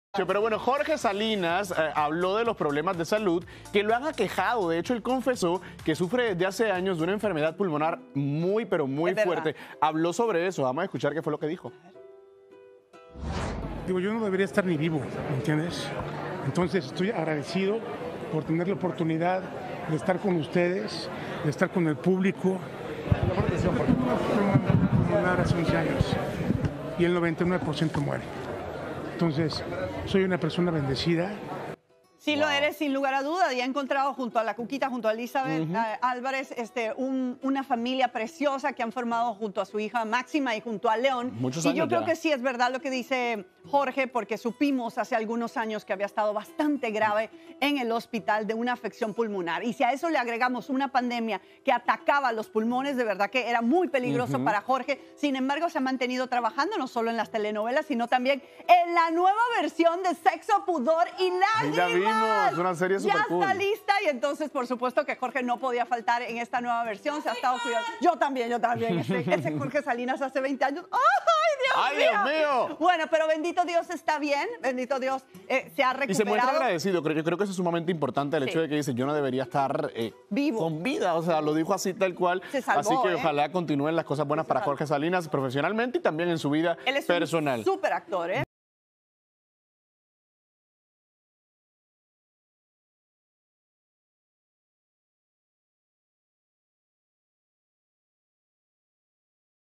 Jorge Salinas confiesa que sufrió grave enfermedad pulmonar | Telemundo
El actor dijo que está agradecido por poder seguir trabajando y compartir junto a su familia y el público.